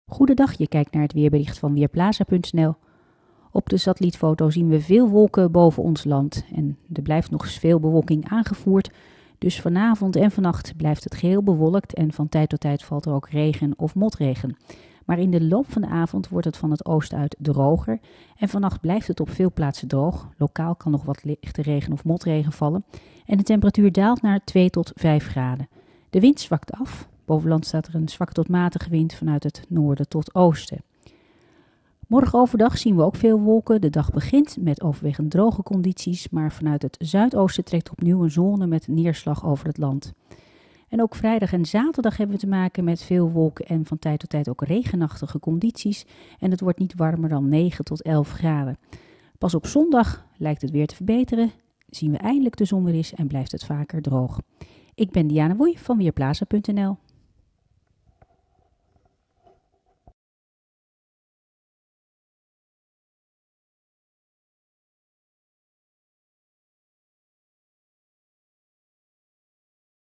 Weerbericht voor woensdagmiddag: Weinig verandering
Het is bewolkt en op veel plaatsen valt er van tijd tot tijd regen. Vanavond en vannacht wordt het vanuit het oosten tijdelijk droger, lokaal kan nog wat regen of motregen vallen en de temperatuur daalt naar zo'n 2 tot 6 graden. Morgen is het nog steeds bewolkt. Vanuit het oosten volgt een nieuw gebied met regen waarmee in de loop van de middag heel het land te maken krijgt. Het wordt 7 tot 12 graden. Ook vrijdag is het regenachtig. Het wordt dan wel iets warmer met 8 tot 13 graden. De zon komt pas in het weekend tevoorschijn.